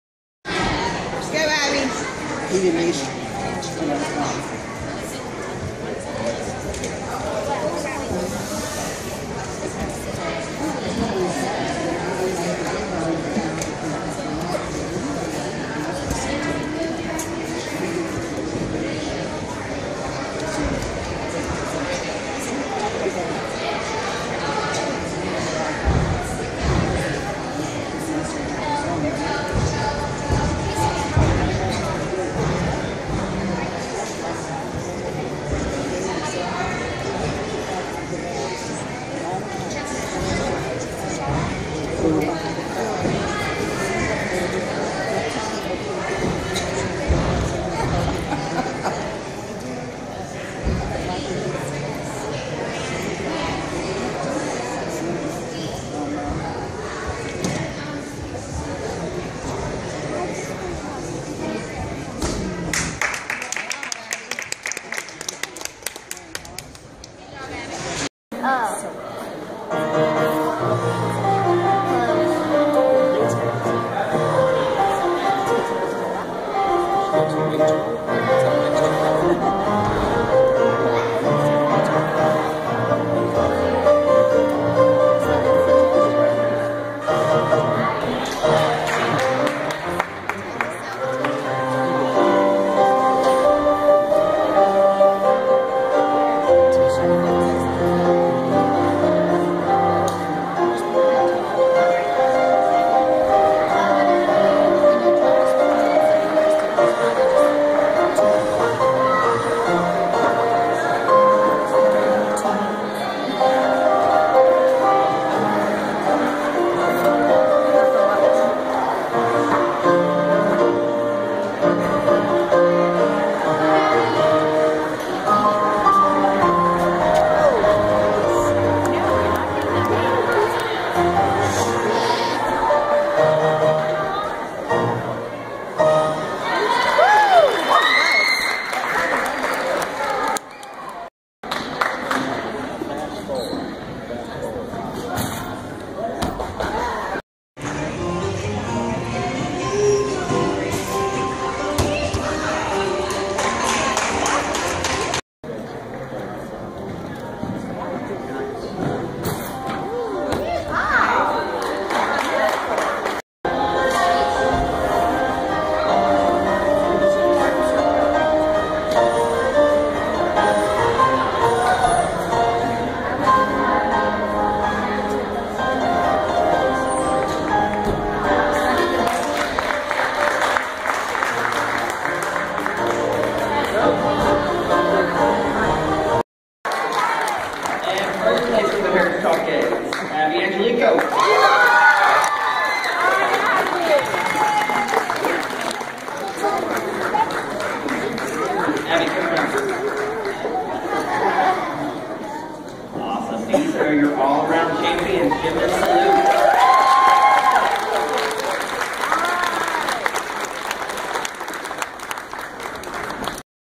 02-15-2014 LKN Qualifier Meet
1st Place Beam, Bars, Vault and All Around!
  AAU Level 4N, Age Group A
  34.75 total score - a personal best!